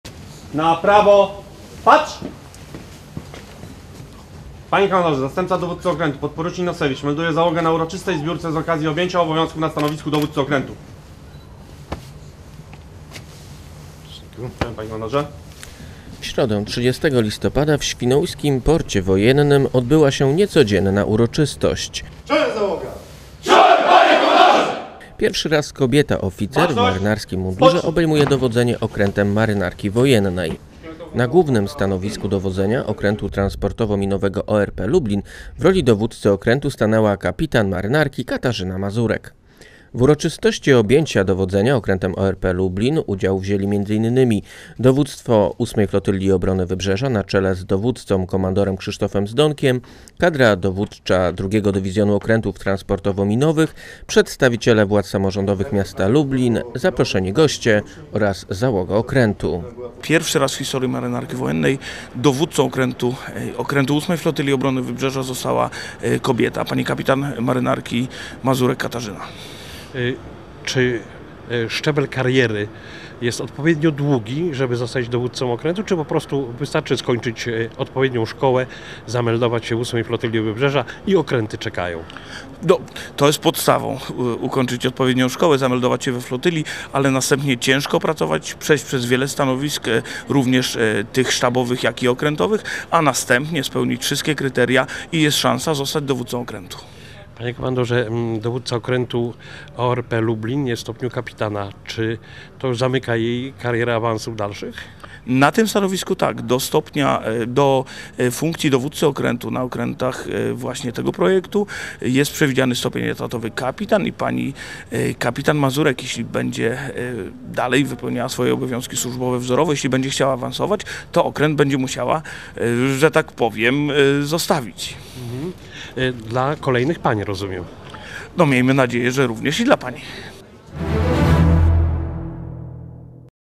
Gdzie diabeł nie może, tam…
Pierwszy raz kobieta oficer w marynarskim mundurze obejmuje dowodzenie okrętem Marynarki Wojennej. Na Głównym Stanowisku Dowodzenia okrętu transportowo - minowego ORP „Lublin” w roli dowódcy okrętu stanęła kapitan marynarki Katarzyna Mazurek. W uroczystości objęcia dowodzenia okrętem ORP "Lublin"  udział wzięli m.in. dowództwo 8 Flotylli Obrony Wybrzeża na czele z dowódcą komandorem Krzysztofem Zdonkiem, kadra dowódcza 2 Dywizjonu Okrętów Transportowo - Minowych, przedstawiciele władz samorządowych miasta Lublin, zaproszeni goście oraz załoga okrętu.